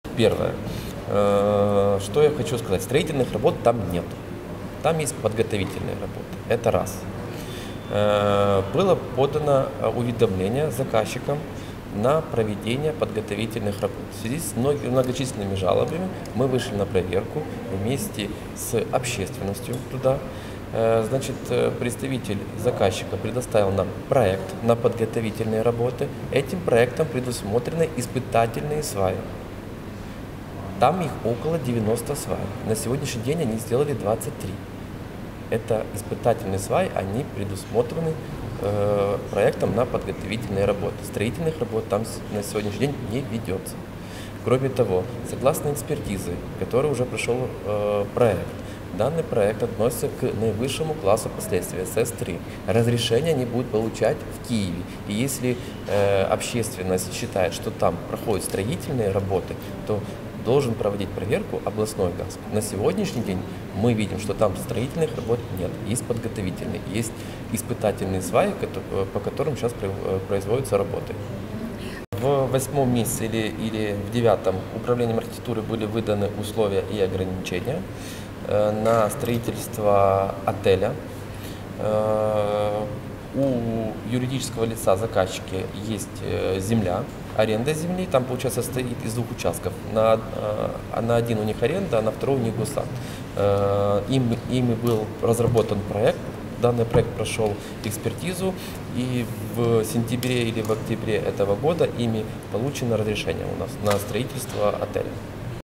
Комментарий городского ГАСК по Гагаринскому плато, 5 и Фонтанской дороге 153-а
По информации управления ГАСК Одесского горсовета по адресу Гагаринское плато, 5 ведутся подготовительные работы. Из 90 испытательных свай, предусмотренных по проекту, сейчас на участке обустроено 23 сваи. Строительные работы на данном участке не зафиксированы. Объект относится а категории сложности СС-3. Разрешения на строительство для таких объектов выдает ГАСИ Украины( г.Киев). Полномочиями на проверку строительных работ на объектах такой сложности наделен ГАСИ Украины в Одесской области.
По адресу Фонтанская дорога, 153-а есть вся необходимая разрешительная документация на строительство отеля.

Комментарий начальника управления ГАСК Одесского горсовета Александра Авдеева